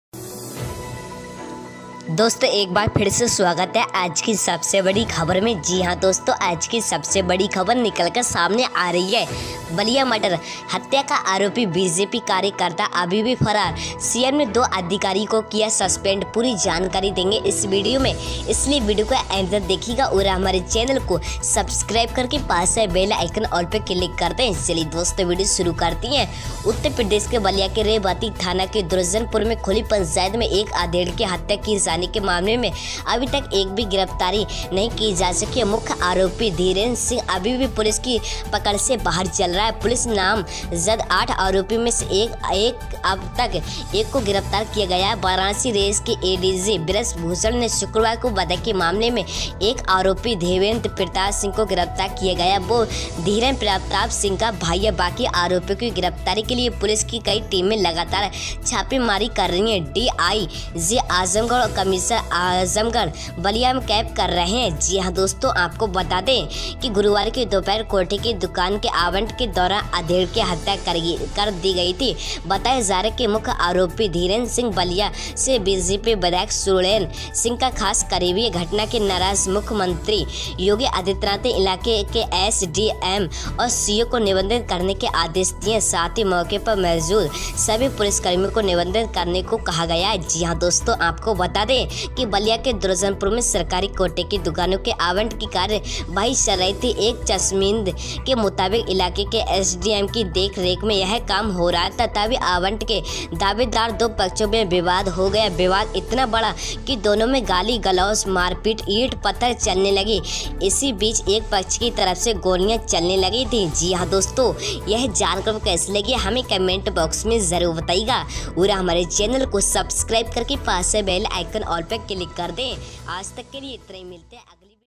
Ballia Murder ll Kote ki dukaan per aadhe ki Hatya
Ballia murder Kote ki dukaan per aadhe ki Hatya. IM News